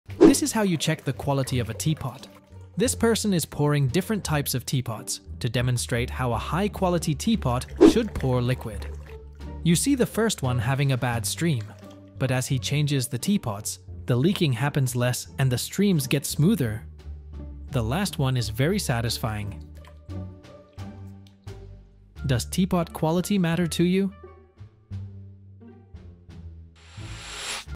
Teapot Quality Check (@Beatwall678-TT)
This person tests the quality of different teapots